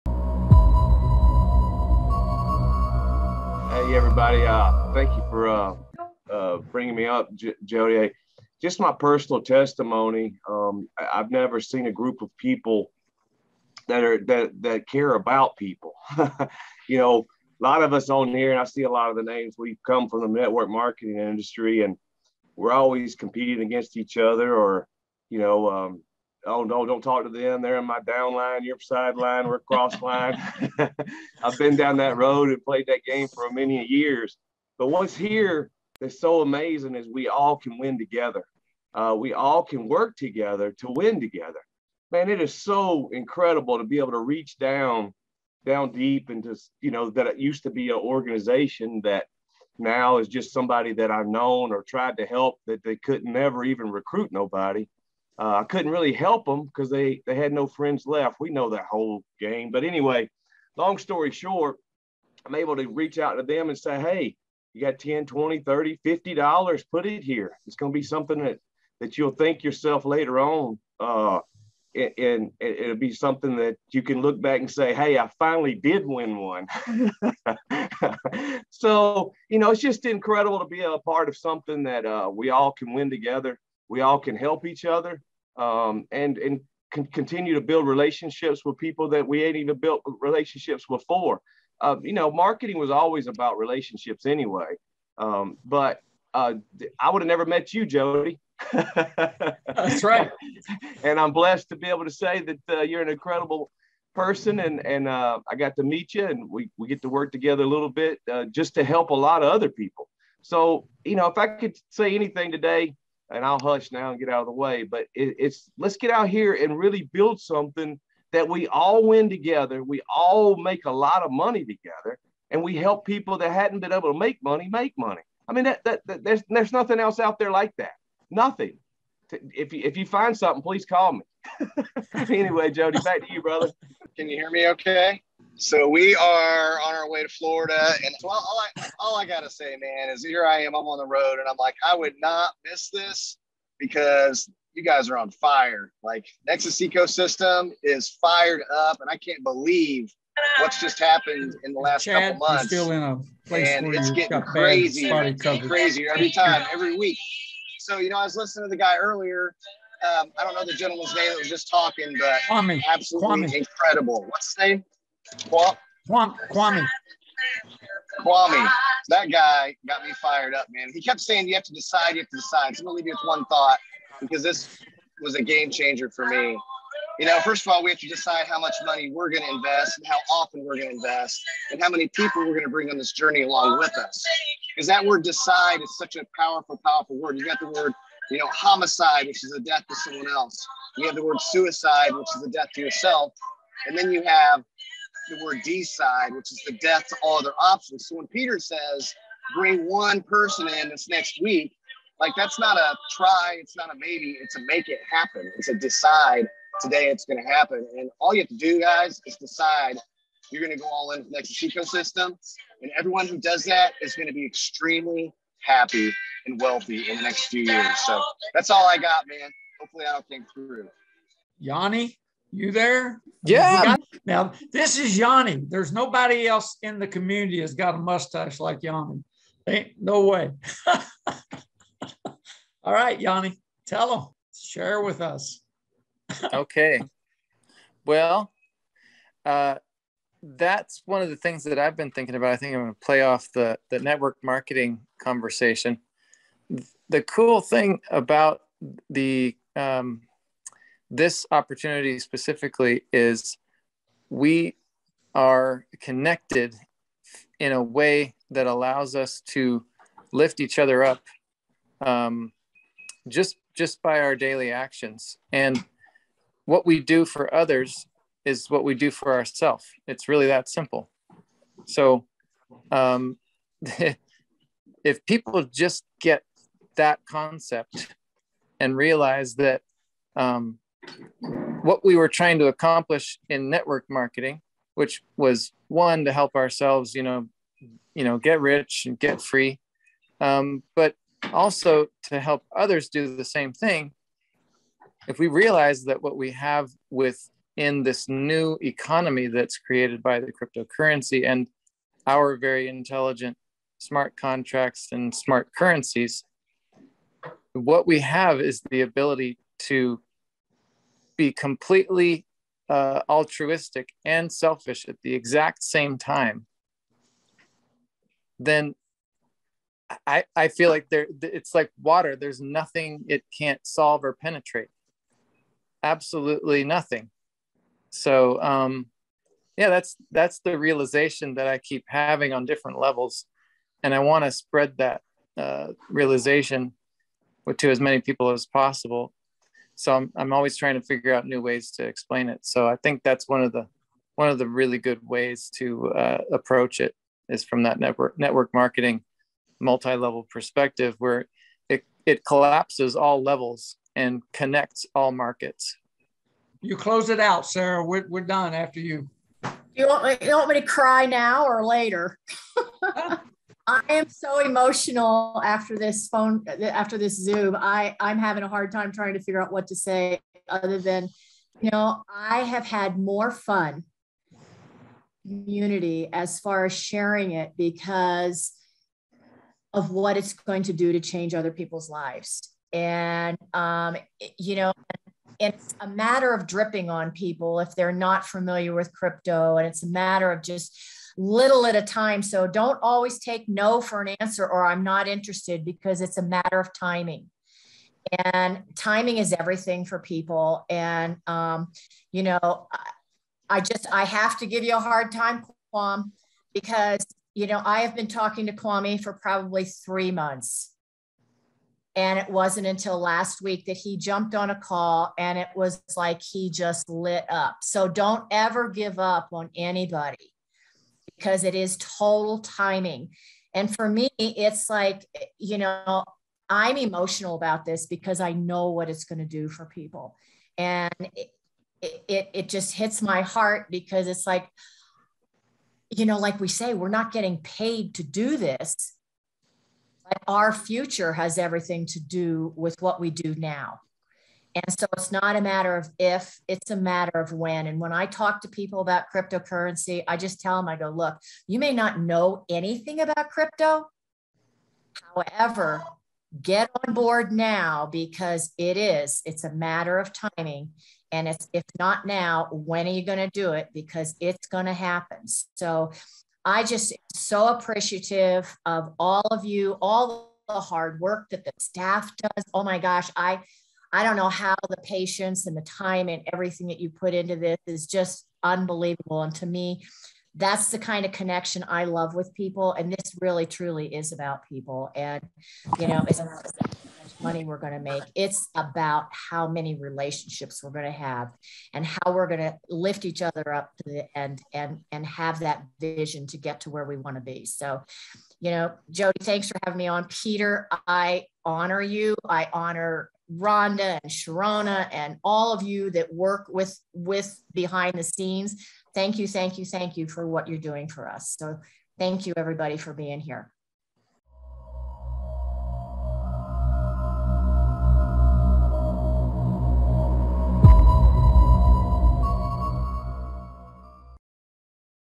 Straight Talk With TruBadger Token Holders ~The Nexus Ecosystem
TruBadger Token holders Joe Jackson, Chad Kneller, Yaan Gulledge, and Sara Warren all give a heartfelt recommendations for getting involved in the TruBadger Nexus Ecosystem! Join the rest of our community on Telegram and while you're there, pick up a couple bags of TruBadger so you too can participate in this movement.